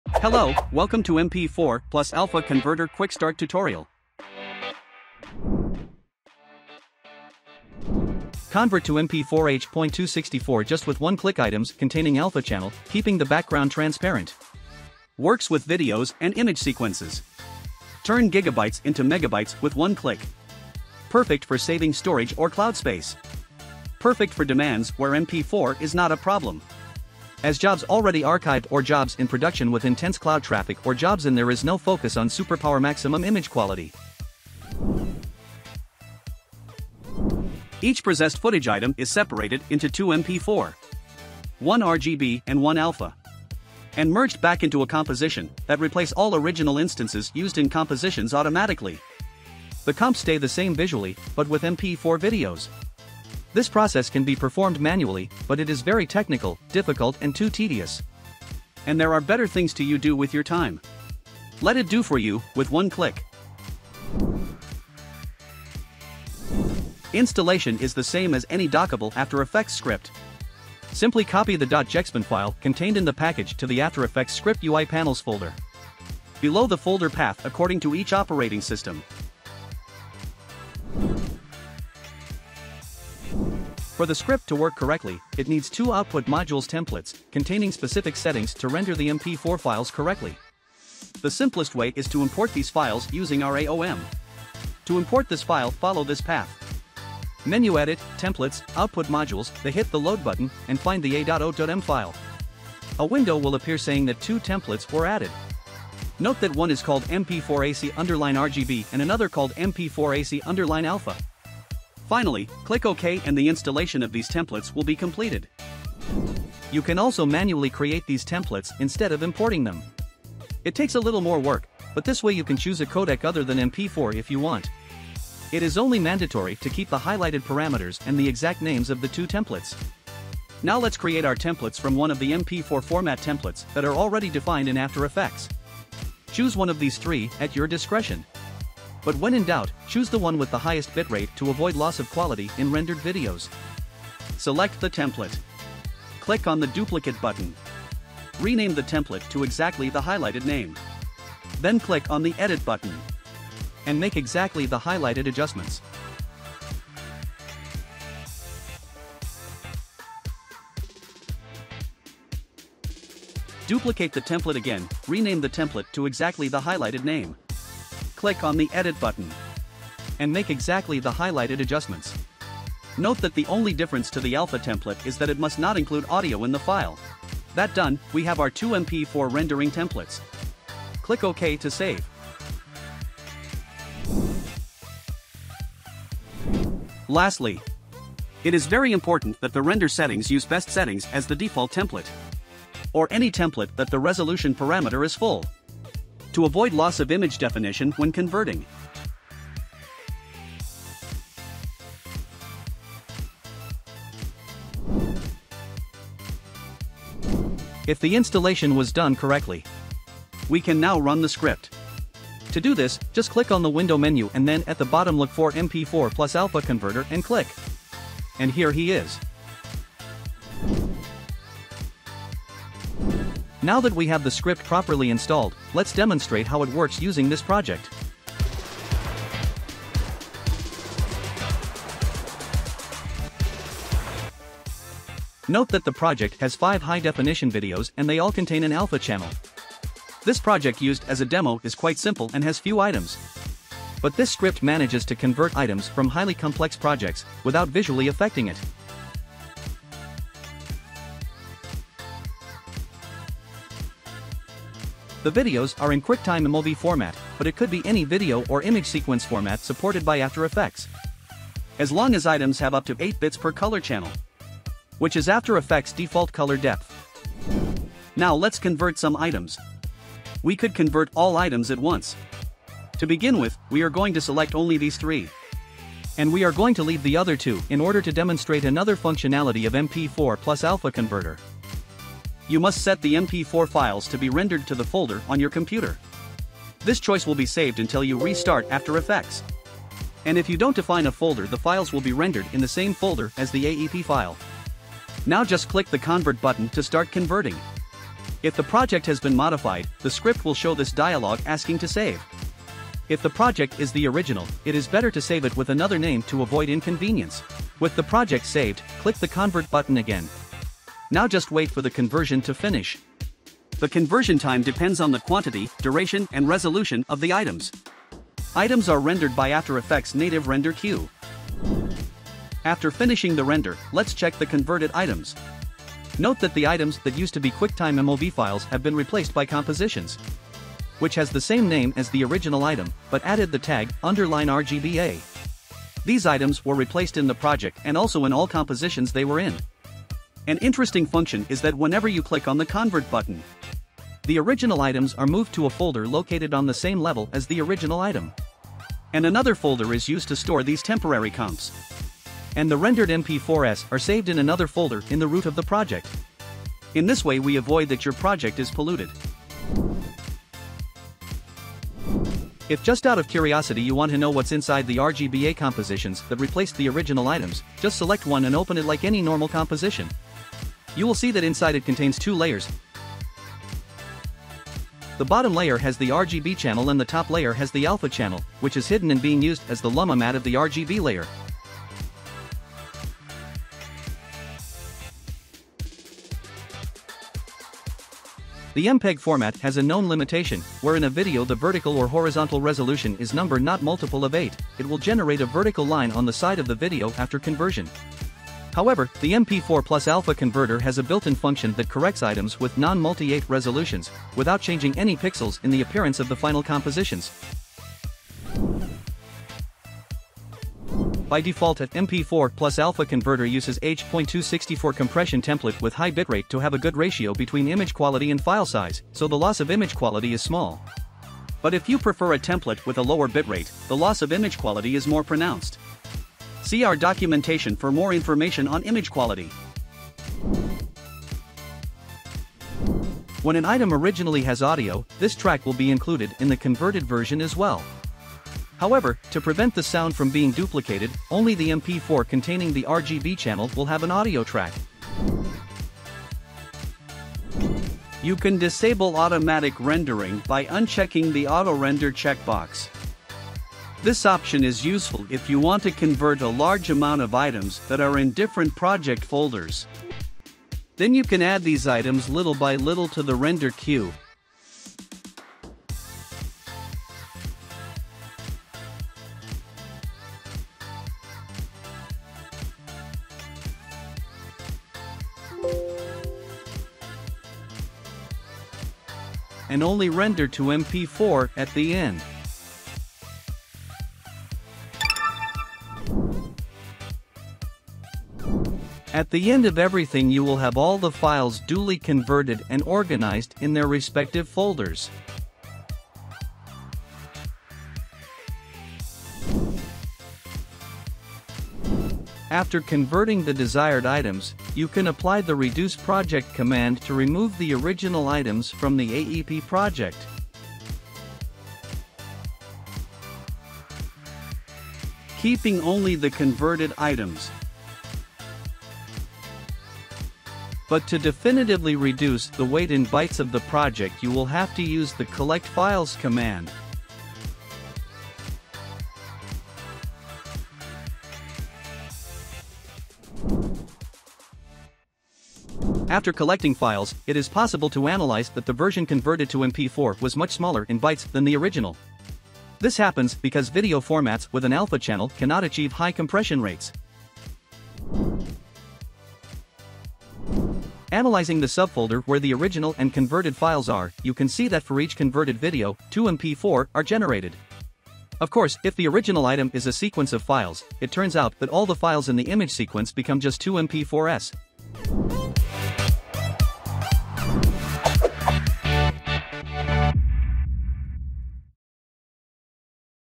MP4 Alpha Converter for After Effects Quick Start
Convert videos with alpha channel to MP4/h264, keeping the background transparent, just with one click.